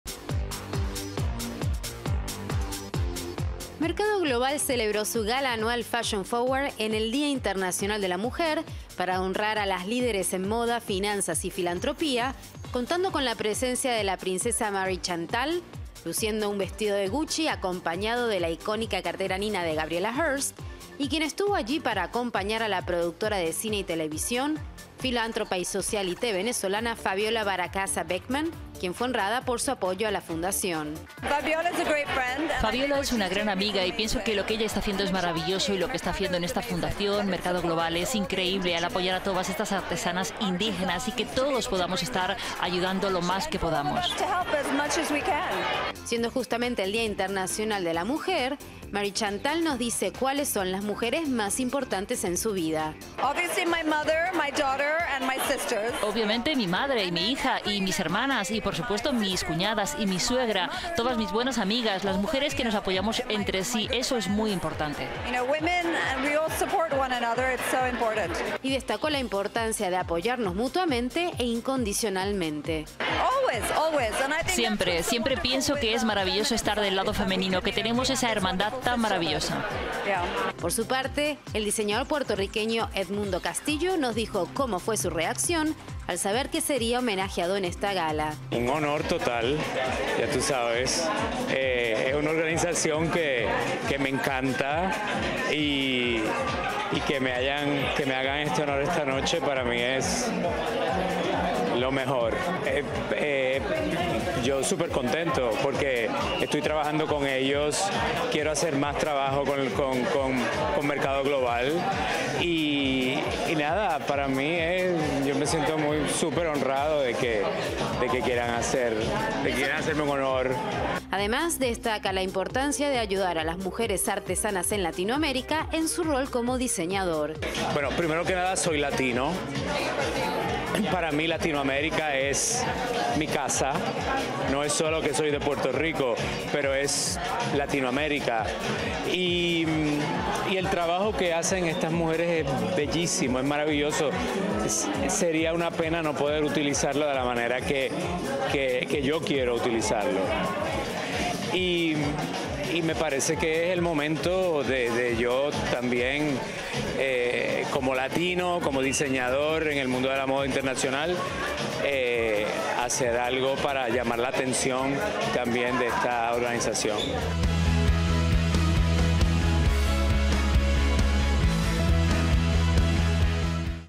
Marie Chantal de Grecia asiste a la gala solidaria de Mercado Global | ¡HOLA! TV
Mercado Global celebró su cita solidaria anual con grandes protagonistas como la princesa Marie Chantal de Grecia en el Día internacional de la mujer. La gala anual Fashion Forward honra a las líderes en moda, finanzas y filantropía, contando con la presencia de Marie-Chantal como protagonista del evento, para el que lució un vestido camisero en blanco y negro de la firma Gucci. La princesa aseguró que estuvo allí para acompañar a la productora de cine y televisión, filántropa y socialité venezolana Fabiola Beracasa Beckman, quien fue honrada por su apoyo a la fundación. Además, reveló cuáles son las mujeres más importantes en su vida.